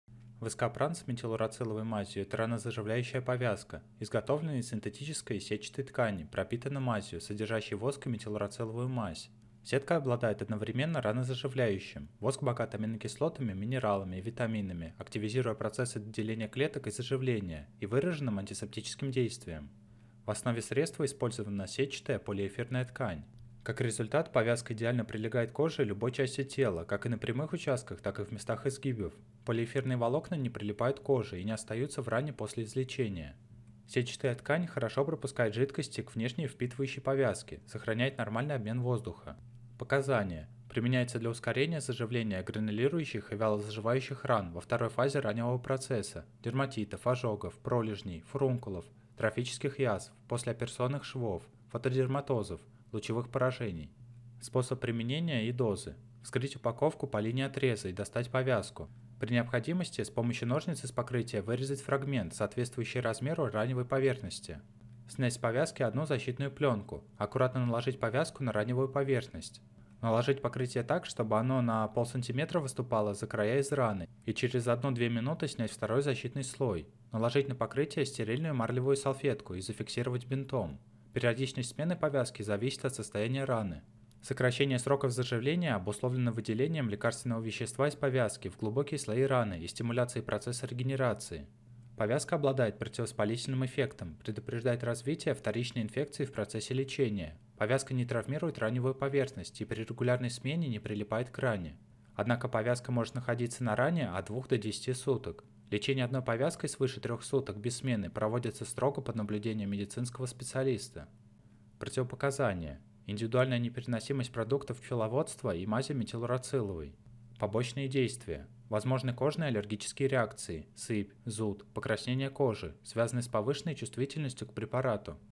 Воскопран мазевая повязка инструкция по применению препарата: Показания, как применять
Воскопран с метилурациловой мазью – ранозаживляющая повязка. Изготовлена из синтетической сетчатой ткани, пропитана мазью, содержащей воск и метилурациловую мазь.

О чём видео:
0:00 Воскопран мазевая повязка
0:39 Показания
0:53 Способ применения и дозы
1:51 Противопоказания
1:57 Побочные действия